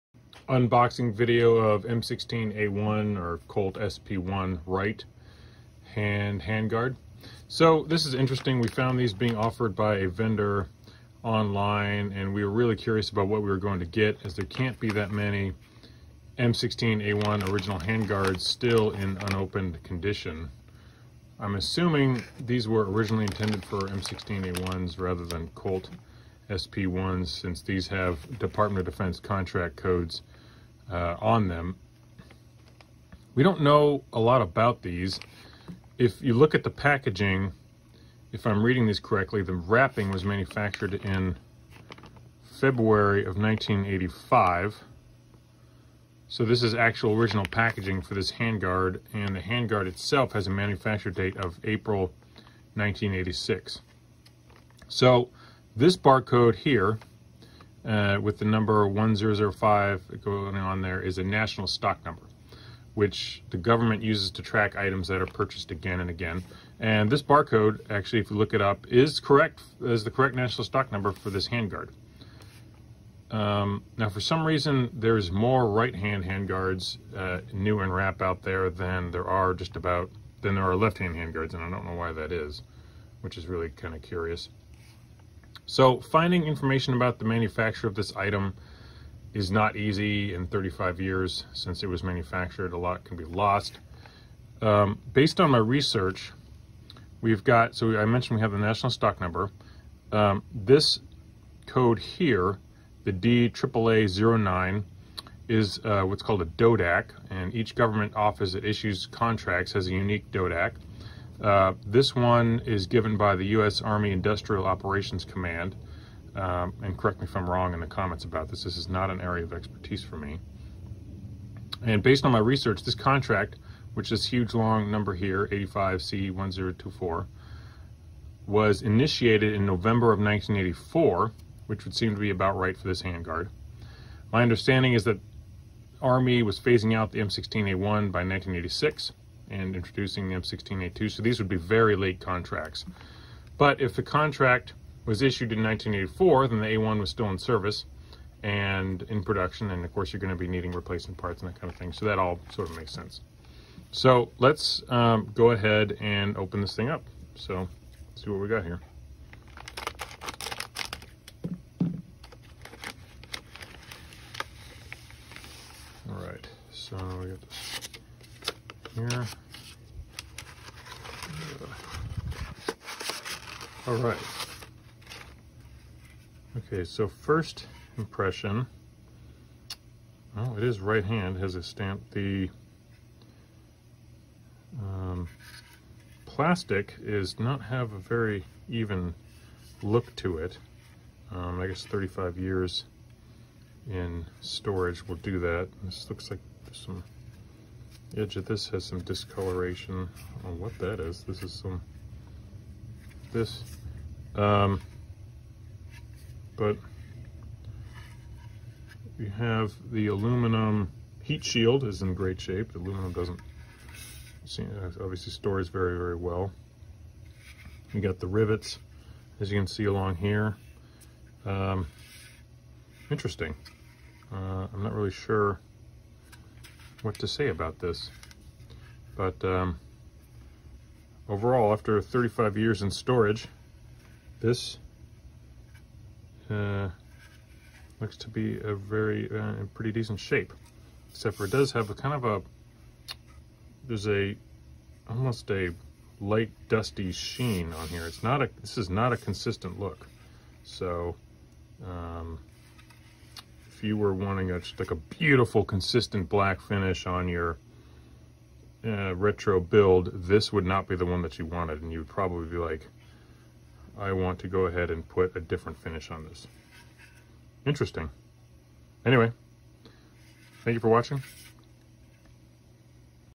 M16A1 Handguard Unboxing | Original Production Retro Handguard
We open a M16A1 Handguard that has been in storage for 35 years.